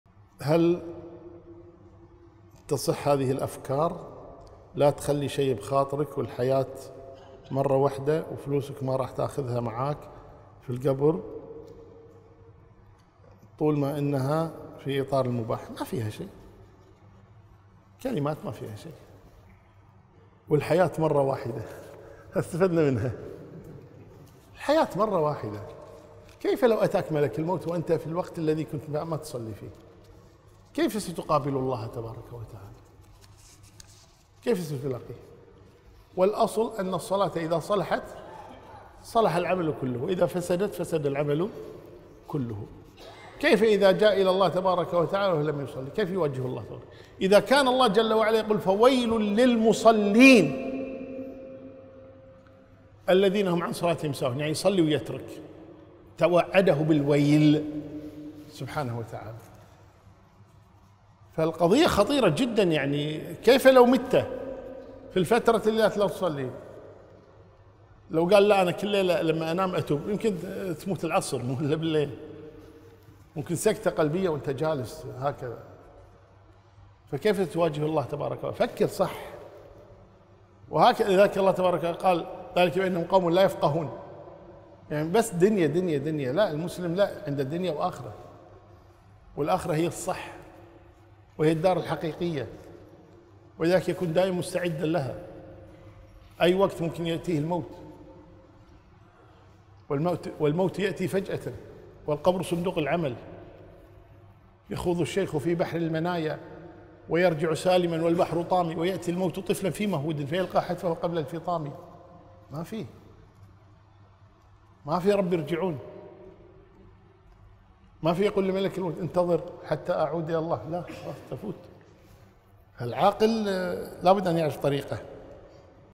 513 - هل يجوز قول (تعيش مرة وحدة فلا تخلي شي في خاطرك)؟ - عثمان الخميس
هذه الفائدة مستخرجة من تفسير سورة الأنفال الآيات ( 61 - 66 )

حسابات الشيخ عثمان الخميس في مواقع التواصل
1_فيس بوك